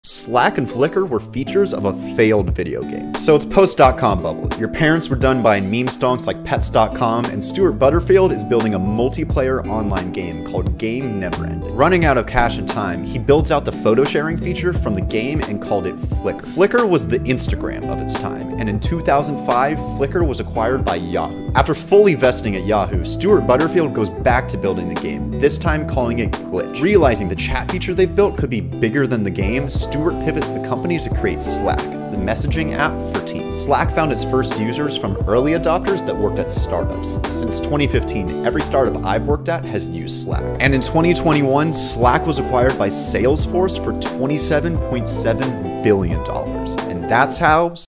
How Slack & Flickr Were Features of a Failed Video Game
Slack & Flickr were features from a failed video game

It’s post dot-com bubble and Stewart Butterfield is building a multiplayer online game called Game Neverending.

Running out of cash & time, he builds out the photo-sharing feature from the game and called it Flickr.

flickr was the instagram of it’s time and in 2005, Flickr was acquired by Yahoo.

After fully-vesting at yahoo, Stewart Butterfield goes back to building the game. This time calling it Glitch.

Realizing the chat feature they’ve built could be bigger than the game, Stewart pivots the company to create Slack: the messaging app for teams.

Slack found it’s first users from early adopters that worked at startups. And in 2021 Slack was acquired by salesforce for $27.7B.